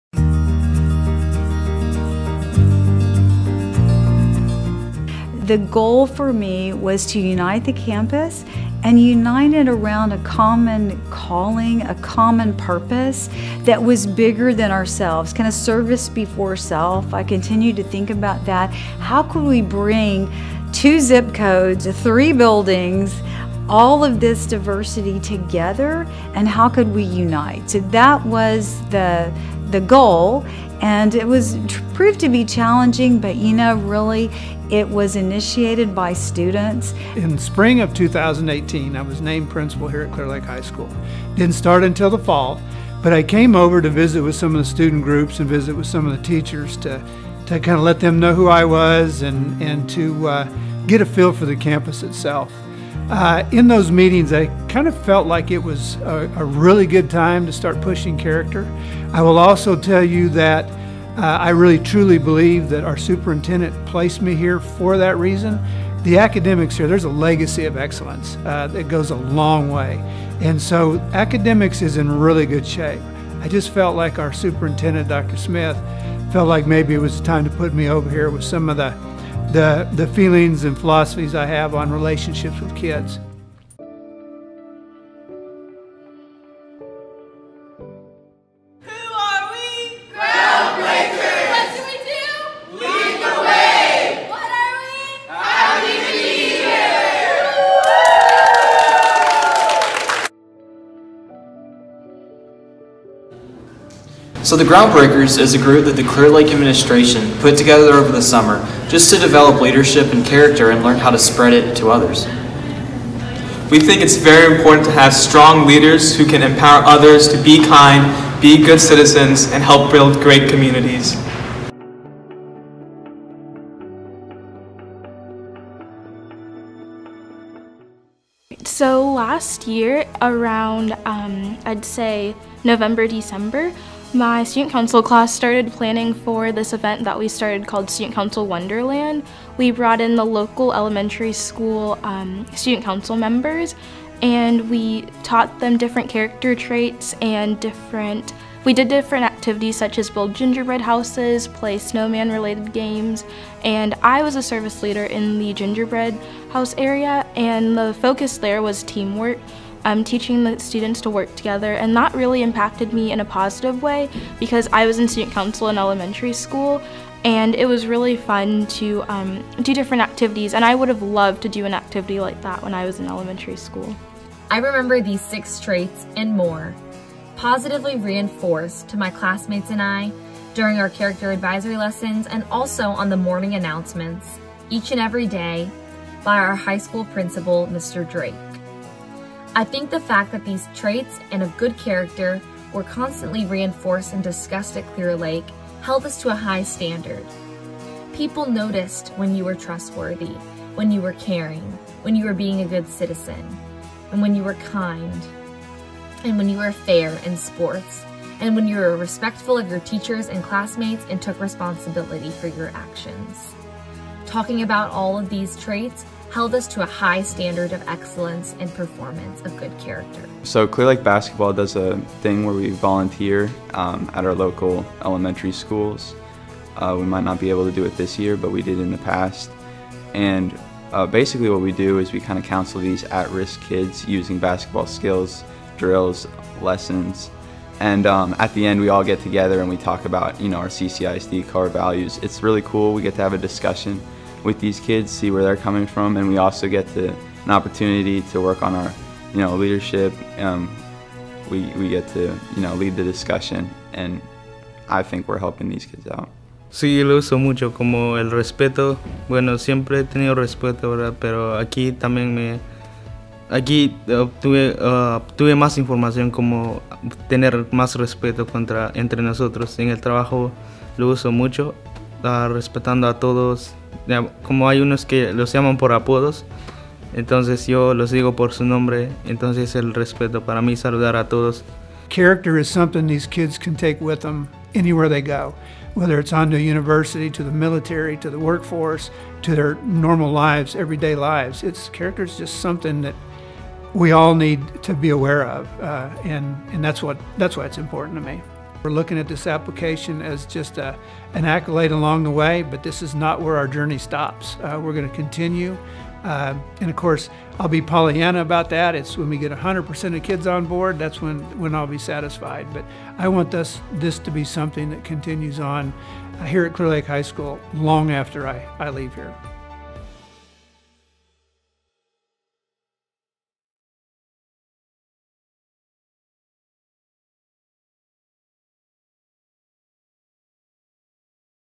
Making Kindness Normal - Clear Lake High School
A campus once divided by zip code, infrastructure, and beliefs is now united by core values and character development. When CLHS underwent a 98-million-dollar renovation, it became evident that the high scholastic expectations embedded deep within its culture, were failing to keep up with the changing demographics of the student population and its surrounding community. The renovation turned a new page by uniting several isolated islands of the campus. Campus leaders recognized significant gaps and began problem solving how they would reach these at-risk students and at the same time unify all stakeholders. Incoming students had learned “The 7 Habits of Highly Effective Teens” beginning the journey. The campus then identified ideal core values and embedded them deep within the essence of the culture.  We began using CharacterStrong, 2Words and Project Wisdom curriculum incorporated into teacher created CLHS advisory lessons. After attending the National Forum of Character, the journey of making character and kindness normal is now an everyday occurrence at CLHS. Looking back at where the old building once stood, long before character was emphasized, exemplifies the dramatic shift in climate that has taken place. A campus with a reputation for high scholastic expectations and creating scholars is now impacting our global community by preparing students to meet the ethical and academic challenges that will arise in everyday life.